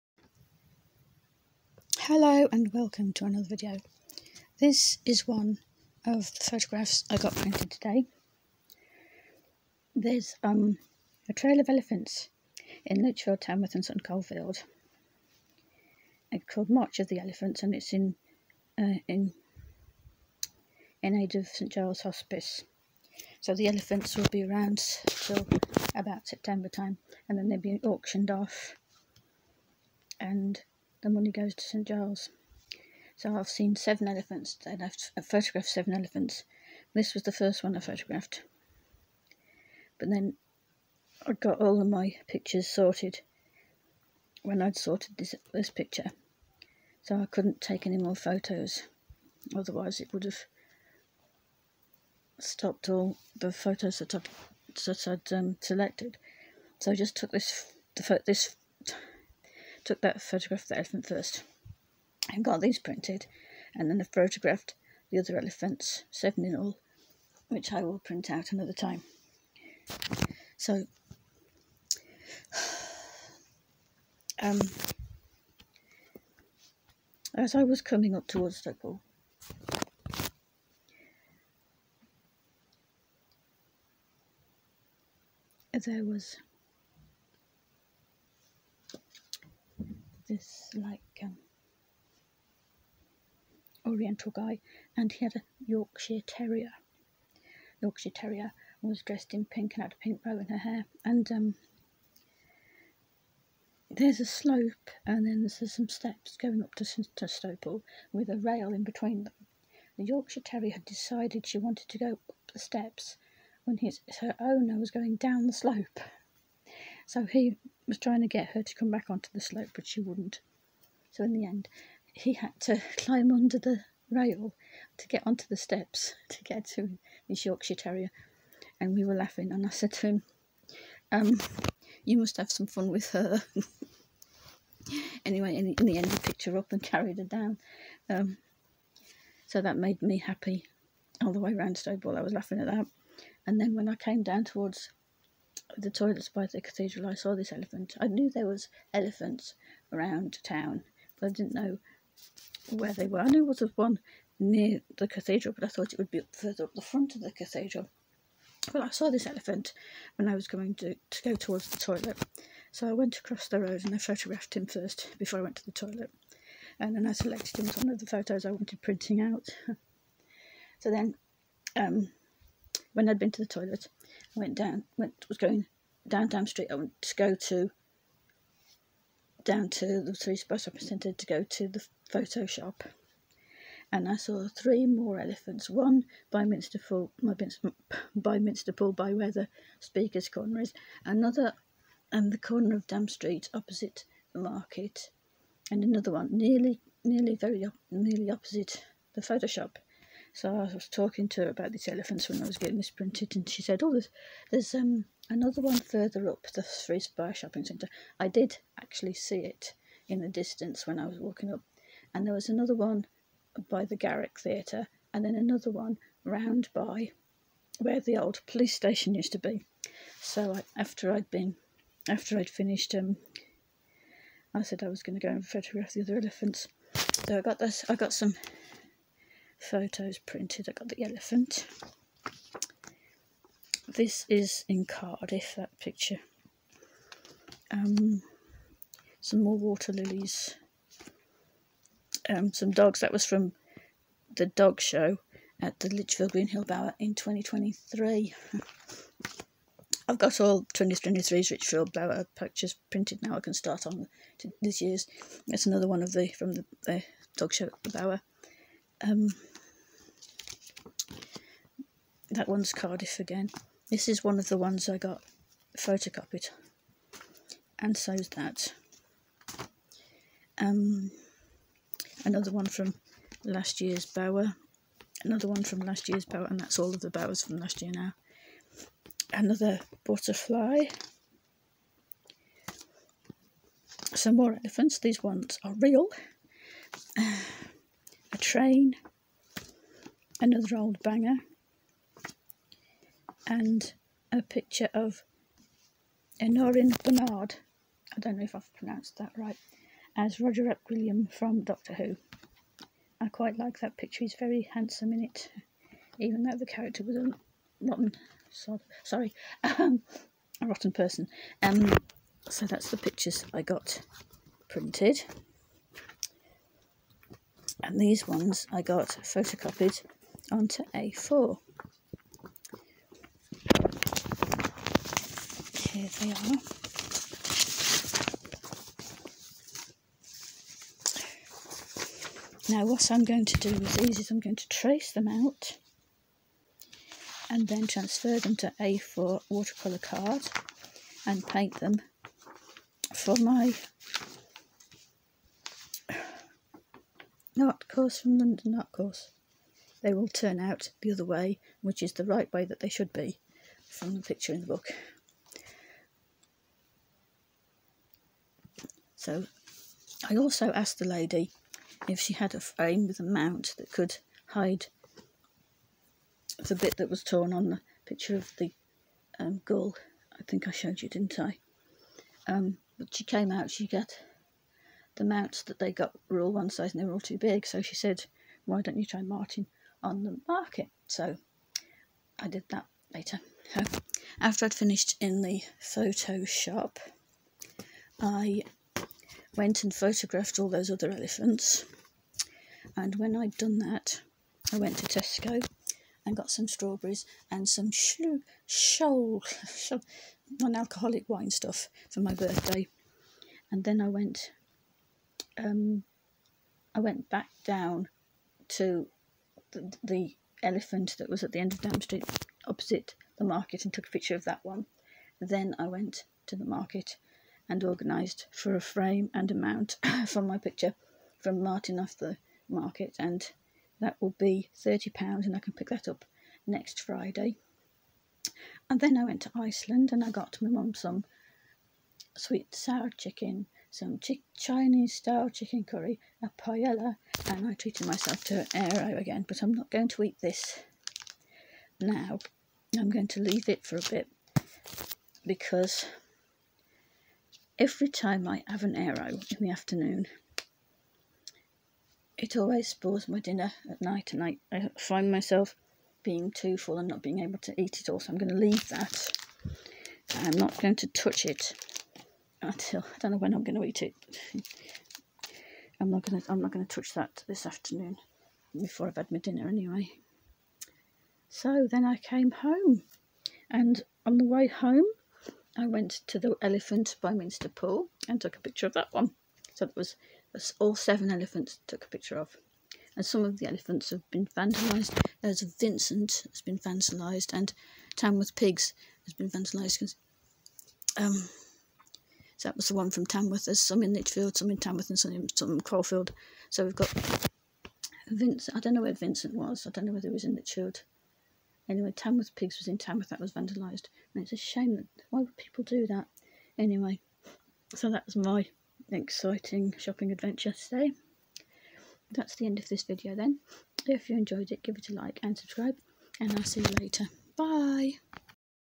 Today's shopping trip down town. (5/7/2024)
In this video I tell you all about my shopping trip down town and show you some of what I got.